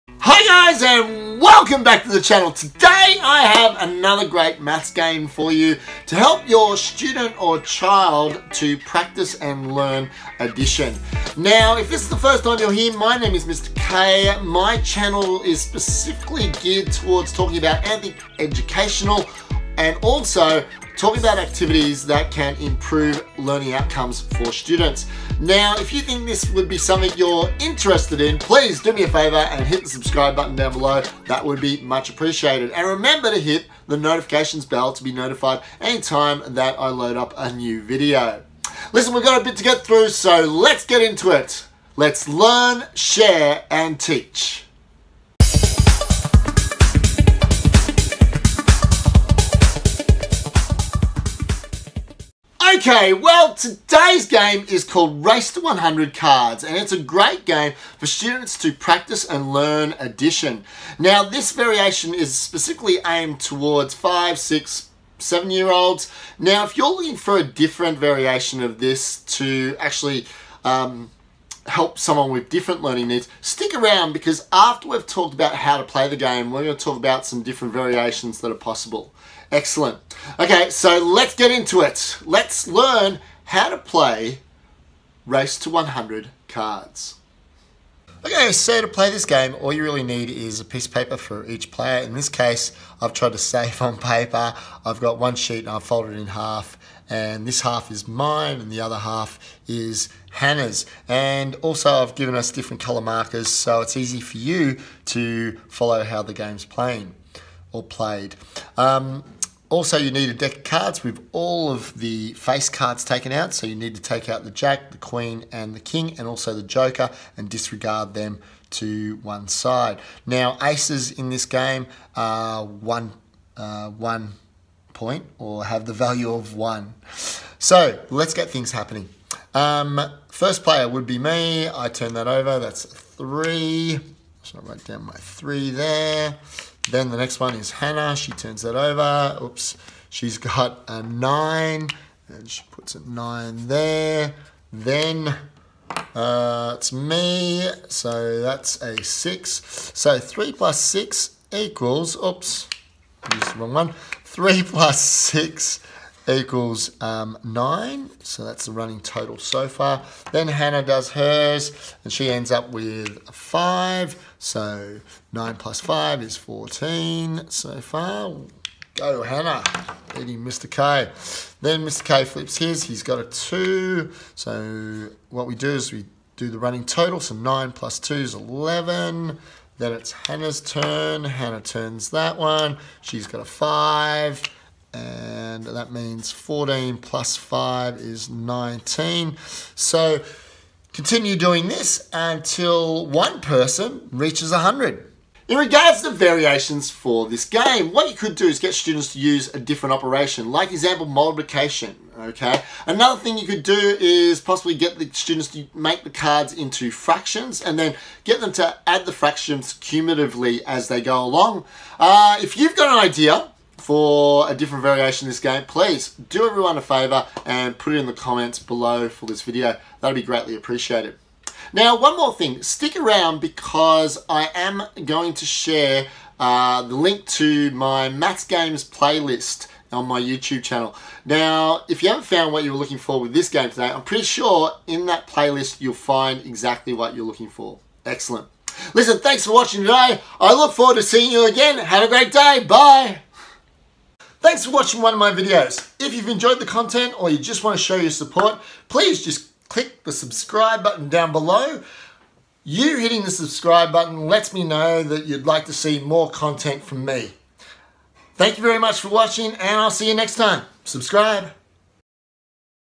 Classroom Math Game - RACE TO 100 CARDS
A great game for students to practise addition between 0-100 and cumulative running totals. Other variations to this game could include subtraction starting from 100 or using higher values. Enjoy!